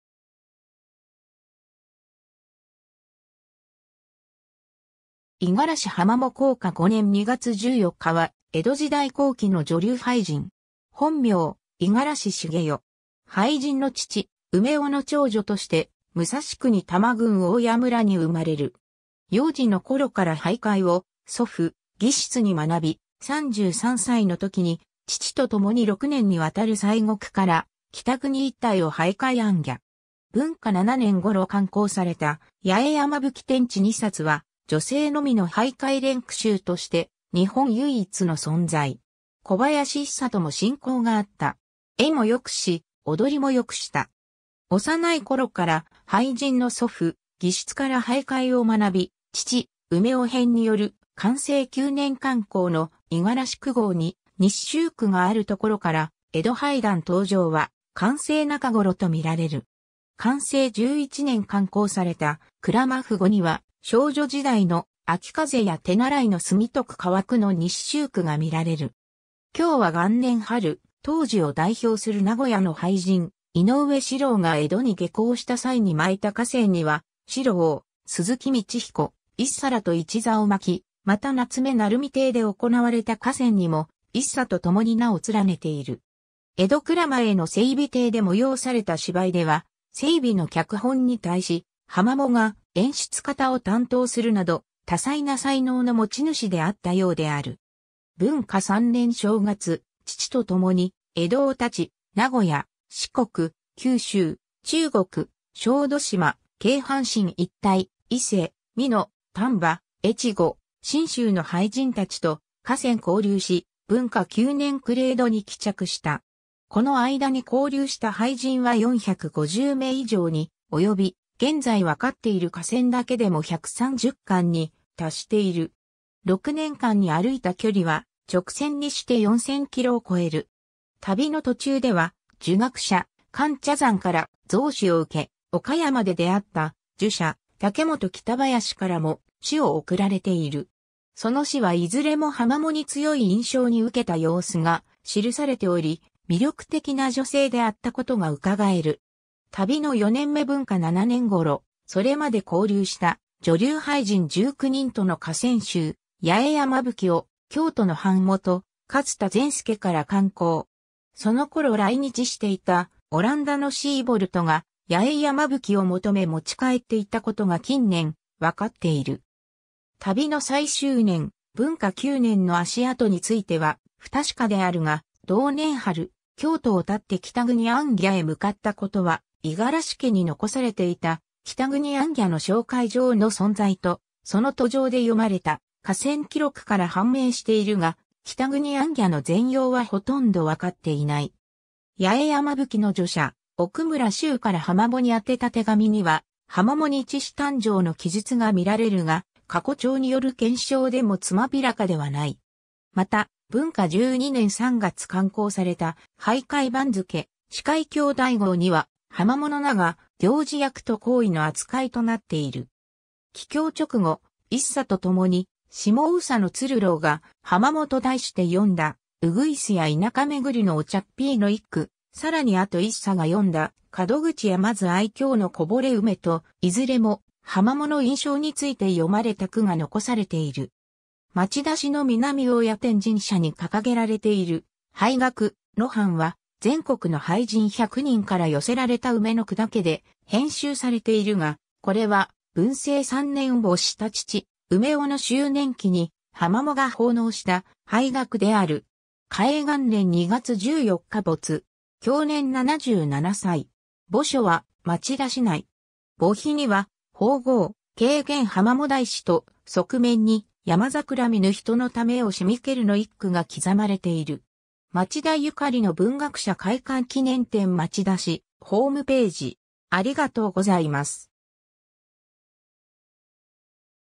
五十嵐浜藻
五十嵐 浜藻（いがらし はまも、安永元年（1772年） - 弘化5年2月14日（1848年3月18日））は、江戸時代後期の女流俳人。
本名・五十嵐茂代。
俳人の父・梅夫の長女として武蔵国多摩郡大谷村（現東京都町田市南大谷）に生まれる。
幼児の頃から俳諧を祖父・祇室に学び、33歳の時に父とともに6年にわたる西国から北国一帯を俳諧行脚。
文化7年（1810年）頃刊行された『八重山吹』（志宇序）天地二冊は、女性のみの俳諧連句集として日本唯一の存在。
小林一茶とも親交があった。
絵も良くし、踊も良くした。
 幼いころから俳人の祖父・祇室から俳諧を学び、父・梅夫編による寛政9年（1797年）刊行の『いがらし句合』に入集句があるところから、江戸俳壇登場は寛政中頃とみられる。
寛政11年（1799年）刊行された『くらま畚（もっこ）』には少女時代の「あきかぜや手習いの墨とくかわく」の入集句がみられる。
 享和元年（1801年）春、当時を代表する名古屋の俳人・井上士朗が江戸に下向した際に巻いた歌仙には、士朗、鈴木道彦、一茶らと一座を巻き（『鶴芝続編』李台編）、また夏目成美邸で行われた歌仙にも一茶とともに名を連ねている。
江戸蔵前の成美邸で催された芝居では、成美の脚本に対し浜藻が演出方を担当するなど、多彩な才能の持ち主であったようである。
 文化3年（1806年）正月、父とともに江戸を発ち、名古屋、四国、九州、中国、小豆島、京阪神一帯、伊勢、美濃、丹波、越後、信州の俳人たちと歌仙交流し、文化9年（1812年）暮江戸に帰着した。
この間に交流した俳人は450名以上に及び、現在分かっている歌仙だけでも130巻に達している。
6年間に歩いた距離は、直線にして4,000キロを越える。
旅の途中では儒学者・菅茶山から贈詩を受け、岡山で出会った儒者・武元北林からも詩を贈られている。
その詩は何れも浜藻に強い印象に受けた様子が記されており、魅力的な女性であったことがうかがえる。
 旅の4年目文化7年（1810年）頃、それまで交流した女流俳人19人との歌仙集『八重山吹』を京都の版元勝田善助から刊行。
その頃来日していたオランダのシーボルトが『八重山吹』を求め持ち帰っていたことが近年分かっている。
 旅の最終年、文化9年（1812年）の足跡については不確かであるが、同年春、京都を発って北国行脚へ向かったことは、五十嵐家に残されていた「北国行脚の紹介状」の存在とその途上で詠まれた歌仙記録（京都舞鶴市郷土資料館蔵の記録）から判明しているが、北国行脚の全容はほとんど分かっていない。
 『八重山吹』の序者・奥村志宇から浜藻に宛てた手紙には、浜藻に一子誕生の記述が見られるが、過去帳による検証でも詳らかではない。
また、文化12年（1815年）3月刊行された俳諧番付『四海兄弟合』には浜藻の名が「行司」役と高位の扱いとなっている。
帰郷直後、一茶とともに下総の鶴老が「浜藻」と題して詠んだ「鶯や田舎巡りのおちゃっぴい」の一句、さらに後一茶が詠んだ「門口やまず愛嬌のこぼれ梅」と、いずれも浜藻の印象について詠まれた句が残されている。
 町田市の南大谷天神社に掲げられている俳額「野斑」は、全国の俳人百人から寄せられた梅の句だけで編集されているが、これは文政3年（1820年）没した父・梅夫の周年忌に浜藻が奉納した俳額である。
 嘉永元年（1848年）2月14日歿。
享年77歳。
墓所は町田市内。
墓碑には法号「瓊厳浜藻大姉」と、側面に「やまざくら見ぬ人のためをしみける」の一句が刻まれている。
 ^ 町田ゆかりの文学者－開館記念展 町田市ホームページ（アーカイブ）